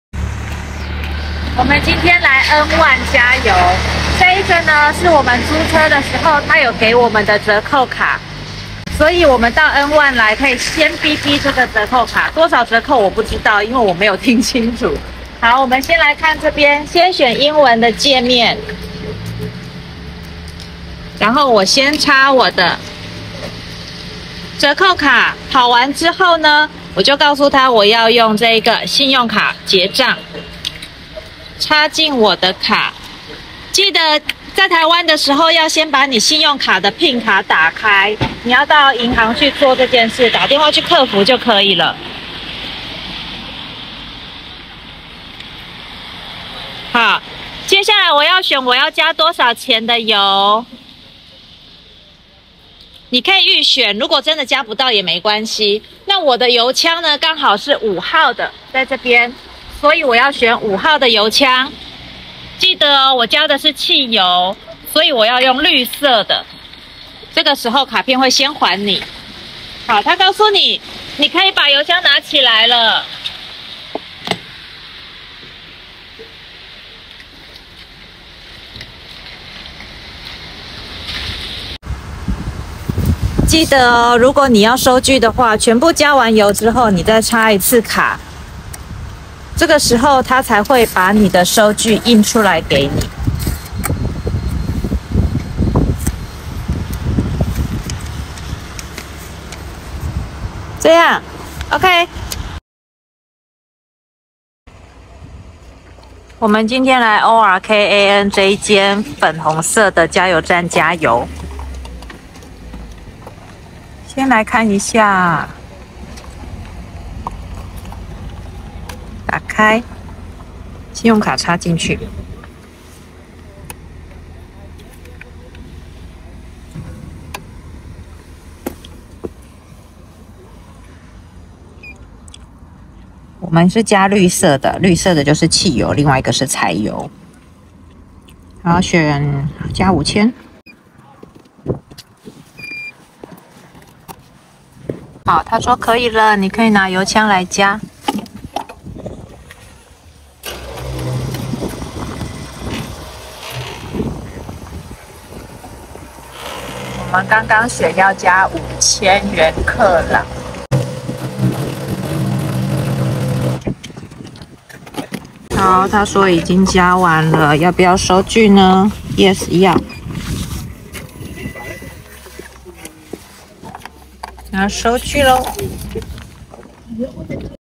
冰島D0-冰島租車自助加油
N1，ORKAN兩家自助加油
記得要先打去銀行開通信用卡的pin碼，在台灣就是預借現金密碼